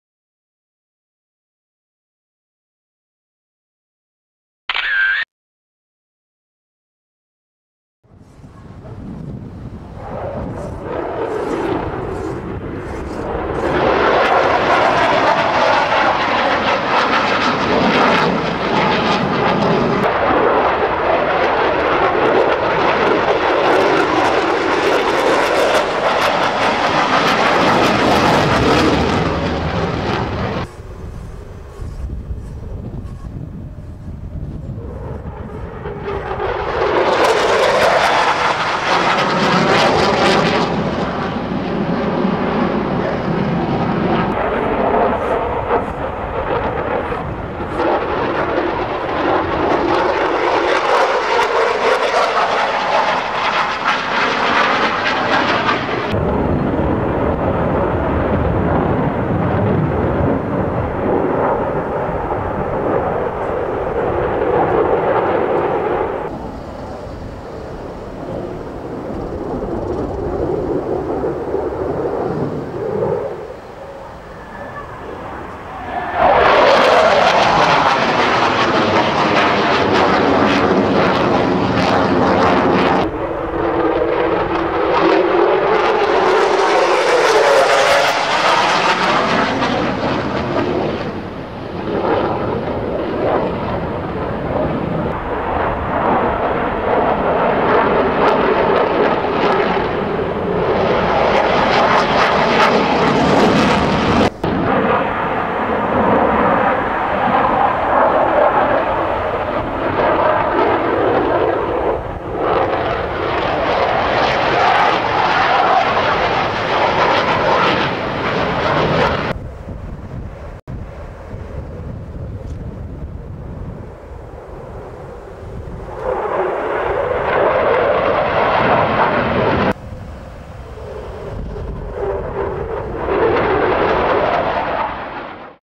Czwartkowy trening ZEUS F-16 DEMO TEAM
Według informacji ze strony pokazów lotniczych był to pierwszy międzynarodowy występ kapitana Emmanuela Karahaliosa bliżej znanego polskiej publiczności jako ZEUS.